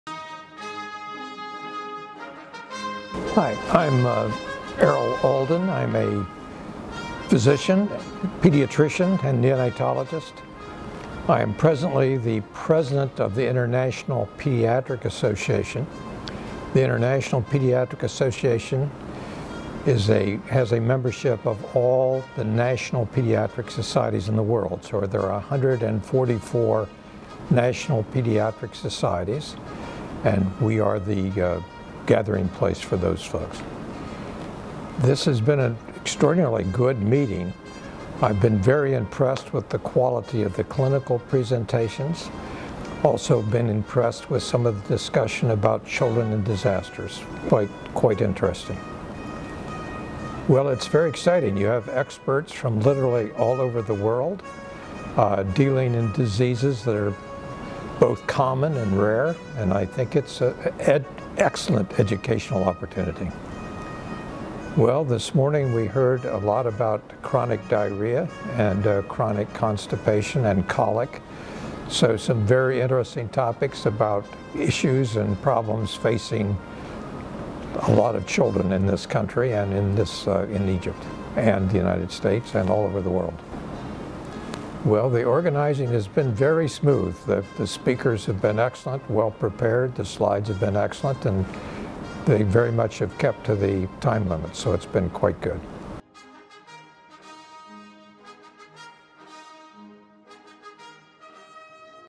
Thank you Professor Errol Alden
Professor Errol Alden,

On behalf of PEDIA EGYPT 2019 CONGRESS, We want to thank you for attending the 7th Annual International congress of pediatrics.

In order to make next year's conference more successful and to exceed the vision, we will be happy to receive your comments and suggestions to improve the next year's conference.

Regards

The 7th Annual International Congress of Pediatrics

📅 Date :- 10 – 13 April , 2019

🌐 venue :- Intercontinental Cairo City Stars Hotel - Cairo , Egypt.